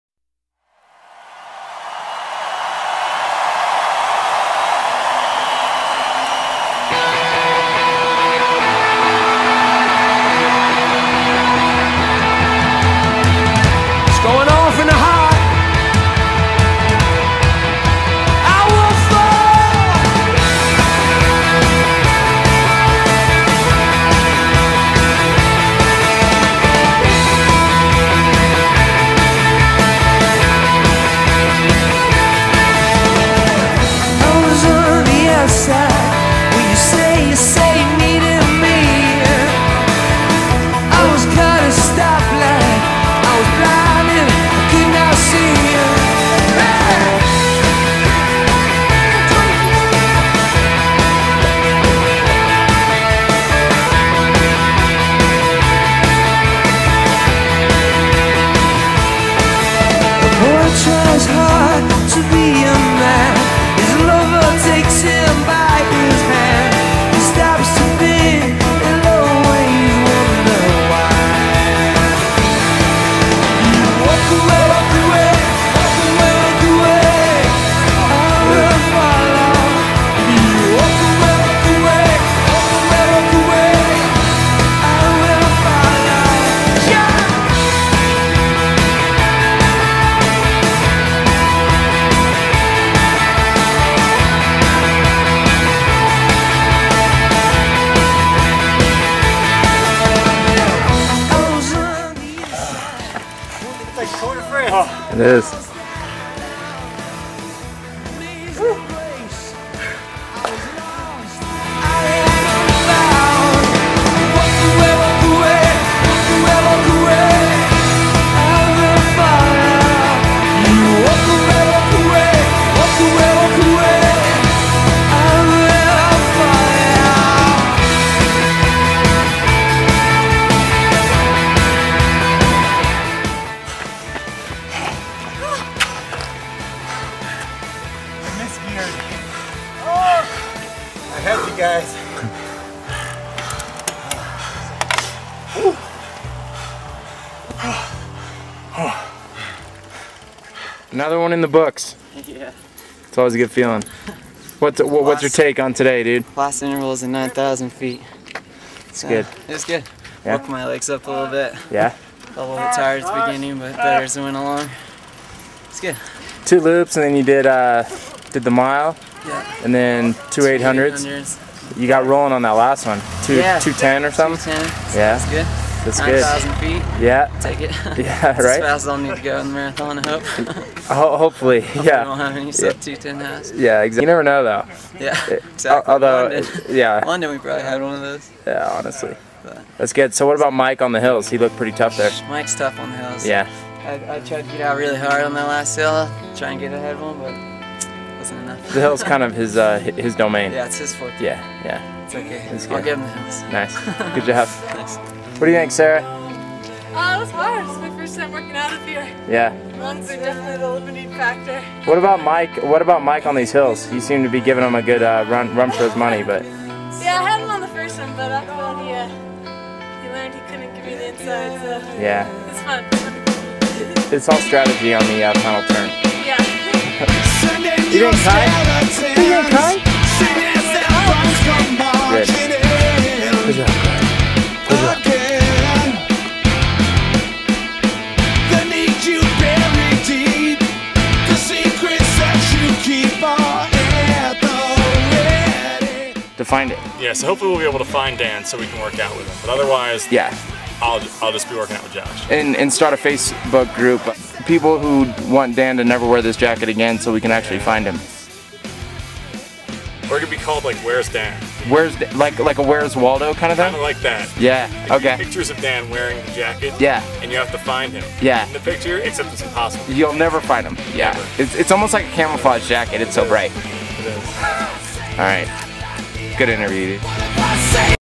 Ryan & Sara Hall Olympic Marathon Hill Workout Mike McKeeman
Mike McKeeman shows off his hill running prowess at 9000 feet. Sara Hall, Ryan Hall, Terrance Mahon, and Kai the wonderdog take it all in.